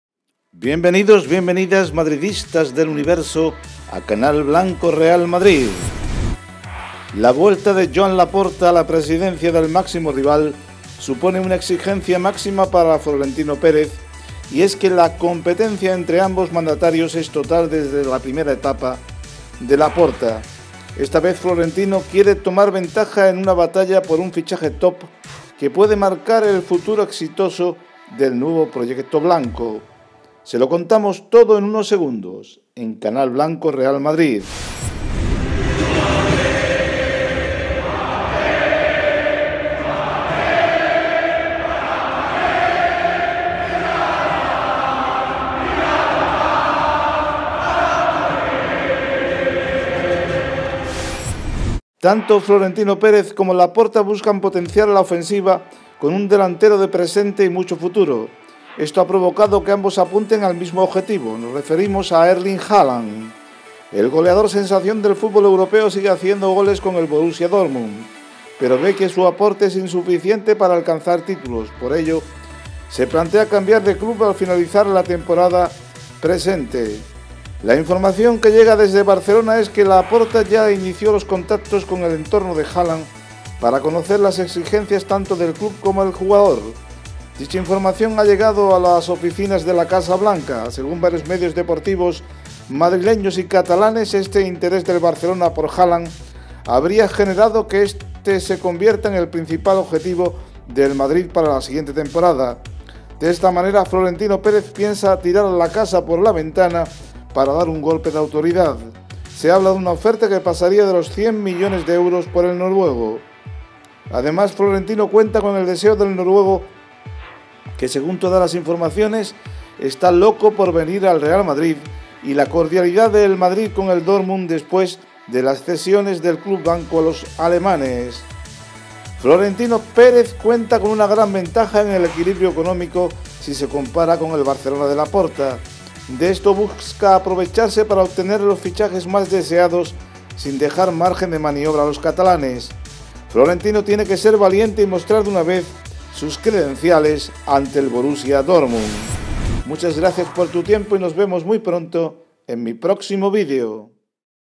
FLORENTINO GANA LA BATALLA A LAPORTA EN UN SUPERFICHAJE PARA LA PRÓXIMA TEMPORADA
Fichajazo: Florentino Pérez gana la primera batalla a Laporta. La vuelta de Laporta a la presidencia del Barcelona va a suponer una exigencia máxima para Florentino Pérez. El dirigente madridista quiere ganar la primera batalla y hacerse con un fichaje top.
OS DEJO EL LINK DE NUESTRO NUEVO CANAL DE YOTUBE "MADRIDISTAS DEL UNIVERSO"